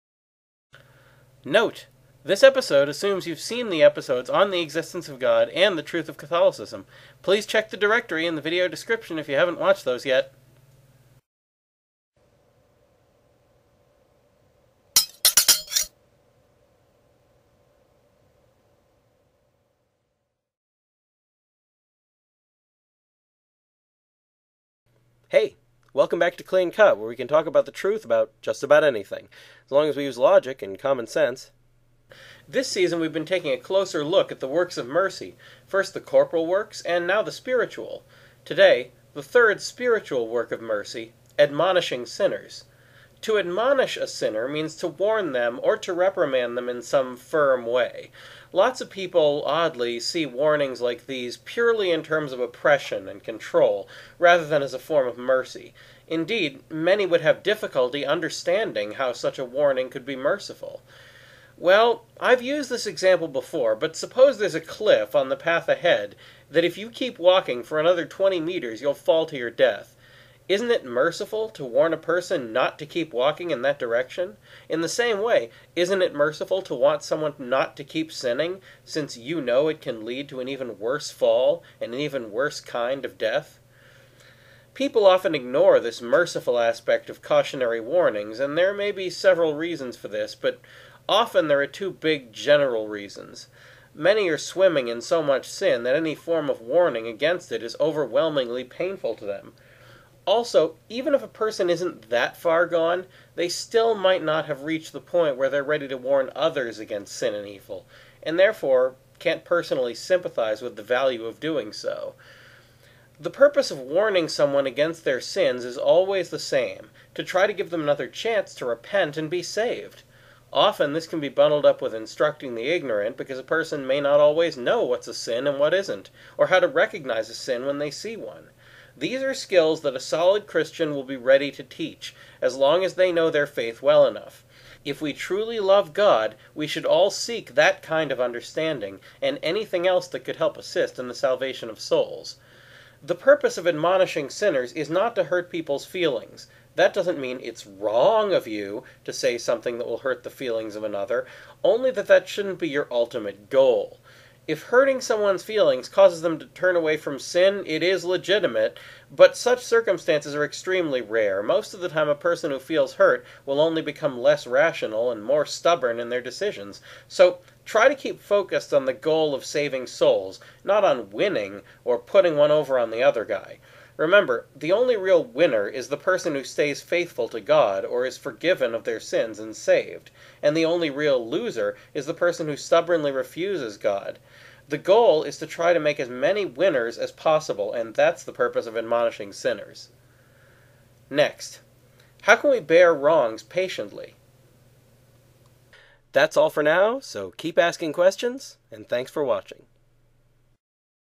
How Should We Admonish Sinners?
What does it mean to admonish someone?  Why should we warn people against sin?  Why do people hate warnings?